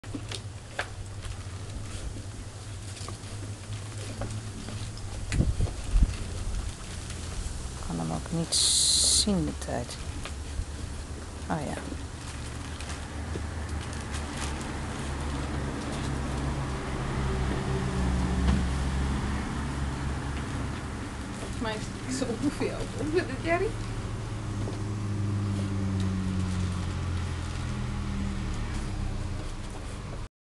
5 pups 23/8 smiddags
De pups 1 uur na de gedragsmedicatie. Voor de medicatie wilde ze amper iets aanpakken, ik kreeg de medicatie er net in met een beetje vlees. En dan doken ze weer weg. Hoewel ze duidelijk wel honger hadden. Nu gaat het vlees er in als ketelaper!